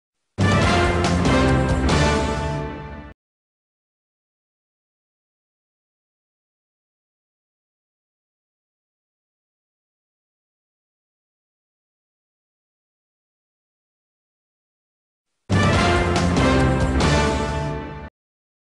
قوات النظام تعتقل أحد المتظاهرين في النويدرات 13/2/2014 Bahrain
قوات النظام تعتقل أحد المتظاهرين خلال تظاهرات خرجت بمنطقة النويدرات صباح اليوم الخميس (13 فبراير/شباط 2014) في أول أيام العصيان المدني الشامل "عصيان العزة" تزامنا مع الذكرى الثالثة للثورة في البحرين.